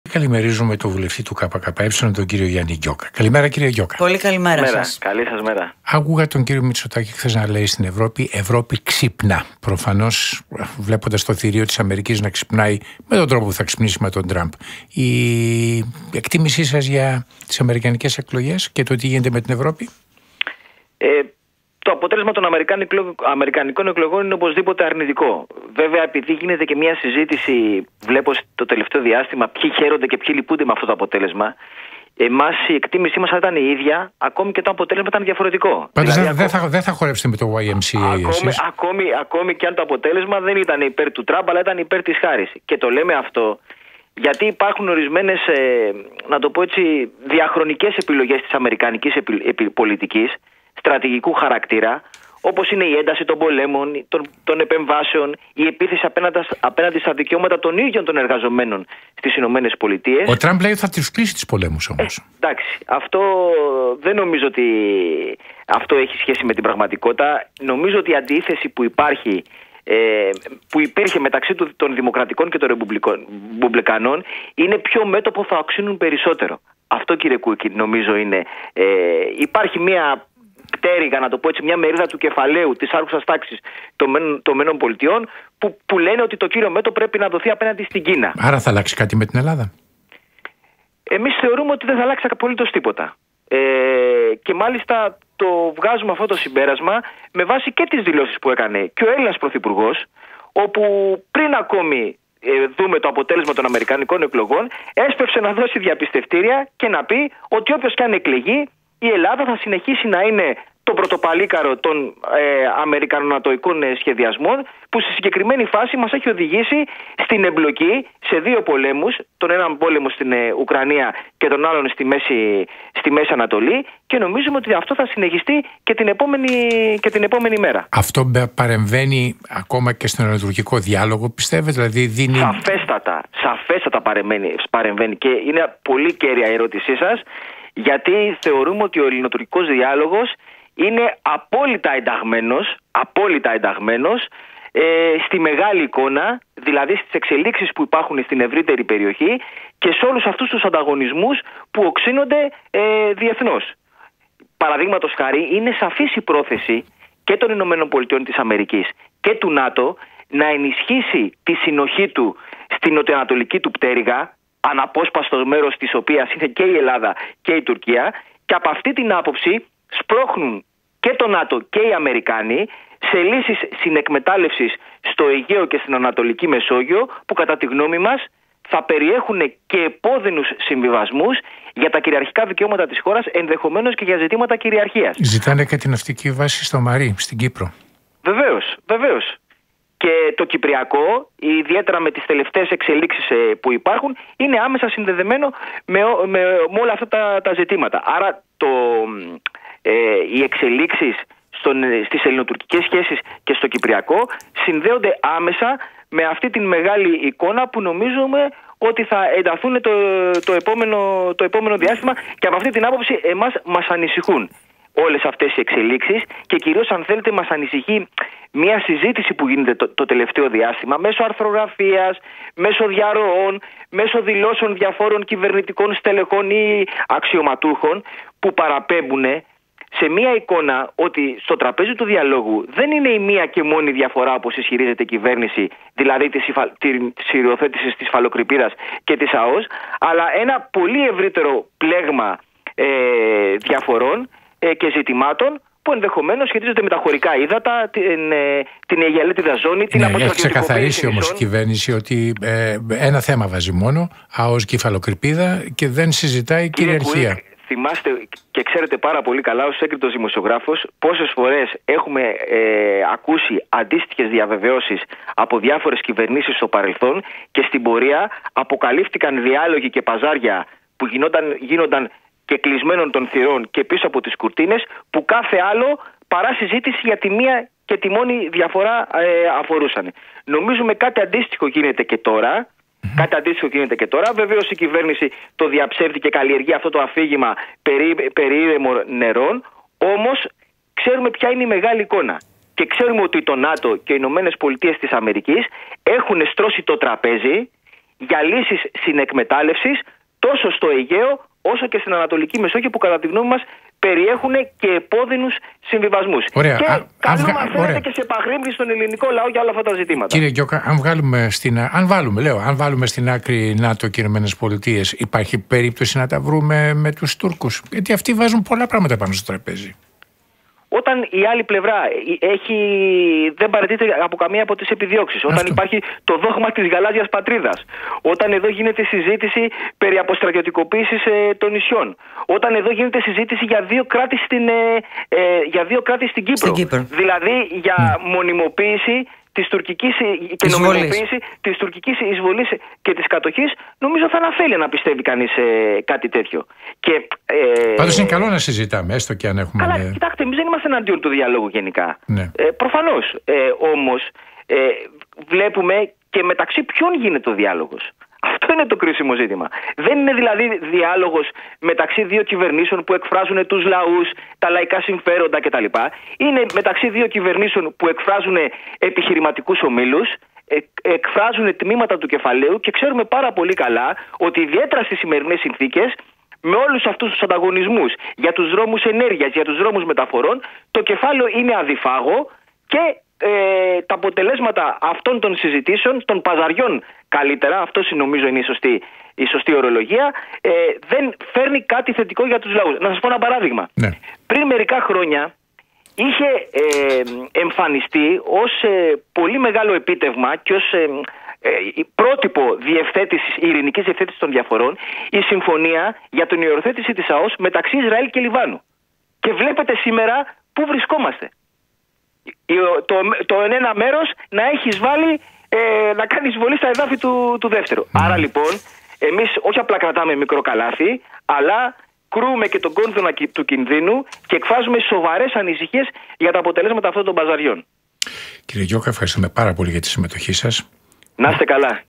Tίποτα θετικό δεν θα φέρουν τα παζάρια στα ελληνοτουρκικά που γίνονται στο ΝΑΤΟϊκό πλαίσιο
Το αποτέλεσμα των αμερικανικών εκλογών είναι αρνητικό, ακόμα κι αν ήταν υπέρ της Χάρις, γιατί  υπάρχουν ορισμένες διαχρονικές επιλογές της αμερικανικής πολιτικής στρατηγικού χαρακτήρα, όπως ένταση πολέμων κι επεμβάσεων κι επίθεση απέναντι στα δικαιώματα των εργαζομένων μέσα στις ΗΠΑ, που δεν αλλάζουν. Αυτό επισήμανε ο *Γιάννης Γκιόκας* βουλευτής βουλευτής του ΚΚΕ μιλώντας σήμερα στο ραδιοφωνικό σταθμό «Real FM»  τονίζοντας πως η αντίθεση μεταξύ ρεπουμπλικάνων - δημοκρατικών ήταν ποιο μέτωπο θα οξύνουν περισσότερο, εξηγώντας ότι υπάρχει μια μερίδα της αστικής τάξης των  ΗΠΑ που θέλει να οξυνθεί το μέτωπο με την Κίνα.

Ερωτηθείς για το αν θα αλλάξει κάτι για την Ελλάδα, σημείωσε ότι δεν πρόκειται ν' αλλάξει τίποτα, με βάση και τις δηλώσεις του ίδιου του Έλληνα πρωθυπουργού, που έσπευσε, πριν ακόμα βγει το τελικό αποτέλεσμα να δώσει διαπιστευτήρια ότι η Ελλάδα θα συνεχίσει να είναι το πρωτοπαλίκαρο των αμερικανο-νατοικών σχεδιασμών, που στη δεδομένη στιγμή,  μας έχει οδηγήσει στην εμπλοκή σε δύο πολέμους, της Ουκρανίας και της Μέσης Ανατολής.

Ο Γ. Γκιόκας σημείωσε ότι μέσα σε αυτή τη μεγάλη εικόνα είναι ενταγμένος και ο ελληνοτουρκικός διάλογος, όπως και στο πλαίσιο των εξελίξεων που υπάρχουν στην ευρύτερη περιοχή με τους ανταγωνισμούς να οξύνονται διεθνώς. Υπογράμμισε ότι «είναι σαφής η πρόθεση των ΗΠΑ και ΝΑΤΟ να ενισχύσουν τη συνοχή του στη ΝΑ Μεσόγειο, μέρη του οποίου είναι και η Ελλάδα και η Τουρκία και γι' αυτό σπρώχνουν σε λύσεις συνεκμετάλλευσης στο Αιγαίο και τη ΝΑ Μεσόγειο που θα περιέχουν επώδυνους συμβιβασμούς για τα κυριαρχικά δικαιώματα της χώρας, ενδεχομένως και για ζητήματα κυριαρχίας. Με αυτά τα ζητήματα είναι άμεσα συνδεδεμένο και το κυπριακό» σημείωσε και πρόσθεσε πως το ΚΚΕ καλεί σε επαγρύπνηση τον ελληνικό λαό.